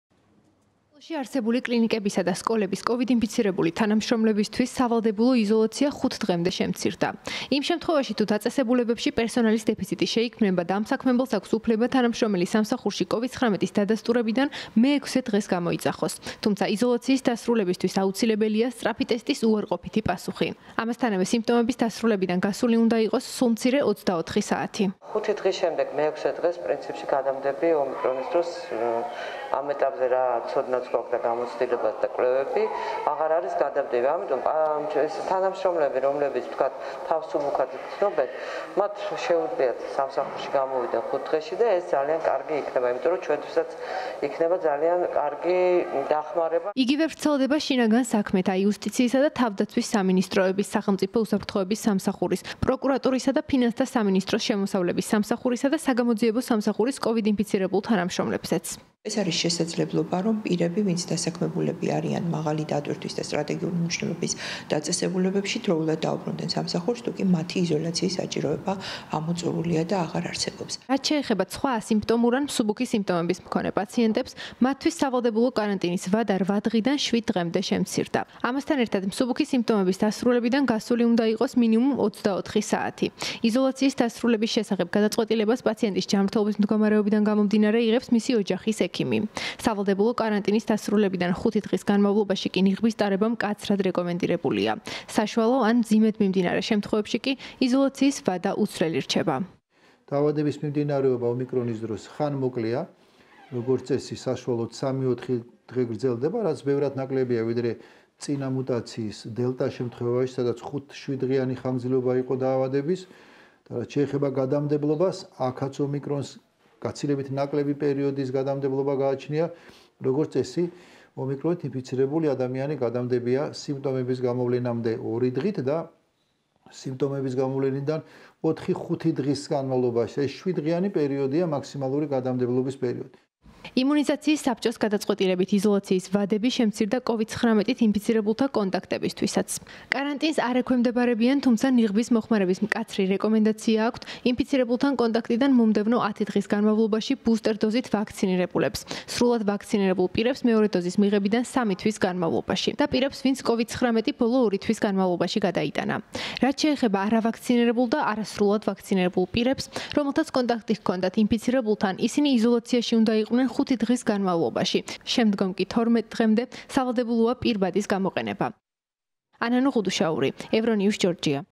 კარანტინის ვადები შემცირდა
COVID-19-ით დაავადებულთა და მათთან კონტაქტირებულთა იზოლაციისა და კარანტინის ვადები შემცირდა